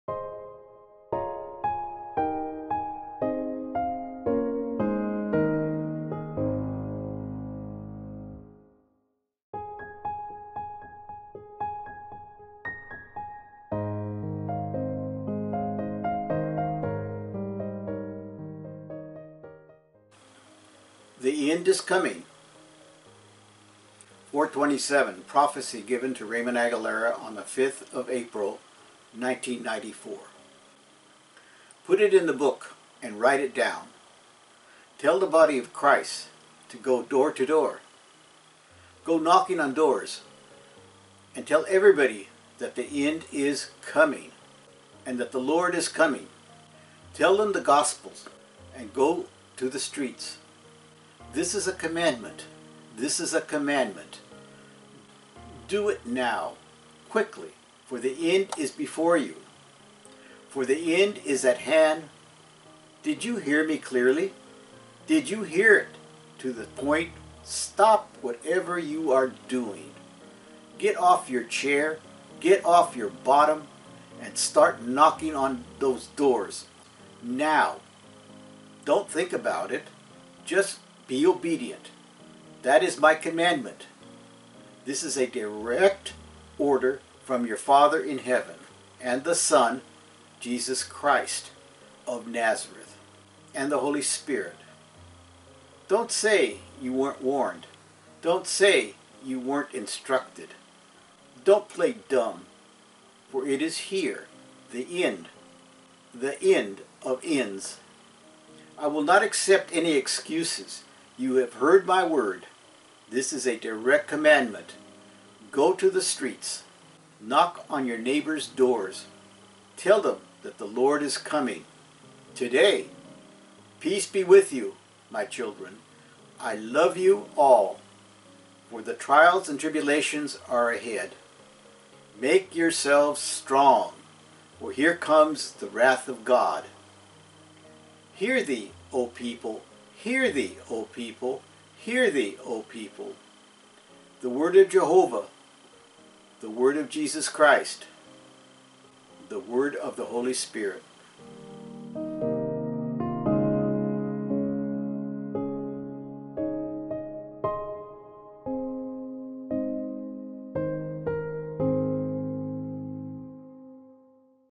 427. Prophecy - The End is coming
Prophecy – Knock on doors and tell everybody the end is coming.

427. Prophecy given to Raymond Aguilera on 5 April 1994 at 9:30 PM. in English.
Put it in the Book and write it down. Tell the Body of Christ to go door to door. Go knock on doors and tell everybody that. The End is coming, and that the Lord is coming. Tell them the Gospels, and go to the streets.
This is a Commandment! This is a Commandment! Do it Now! Quickly, for the End is before you. For the End is at Hand. Did you hear Me Clearly? Did you hear it to the Point? Stop whatever you are doing; get off your chair; get off your bottom, and start knocking on those doors. Now! Don’t think about it, just be obedient. That is My Commandment. This is a direct order from your Father in Heaven, and the Son, Jesus Christ of Nazareth, and the Holy Spirit.
Don’t say you weren’t warned.
(To read the whole Prophecy go to our websites or watch the whole video)
Please Share this with everyone you know - with groups too.